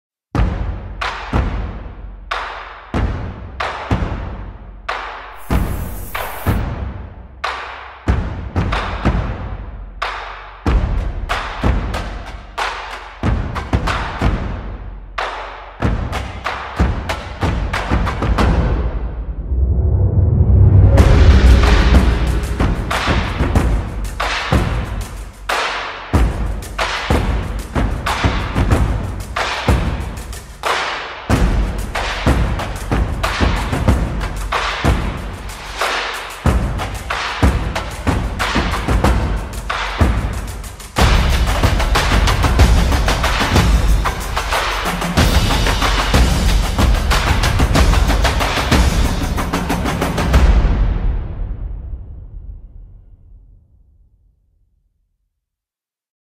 TBG 2020 | New Year, New Toys (iPhone 11 Pro, Moment Anamorphic Lens)
The Basement Gym of St. Charles, IL showcasing some of their new workout equipment.

All footage was captured using an iPhone 11 Pro equipped with the Moment Anamorphic lens.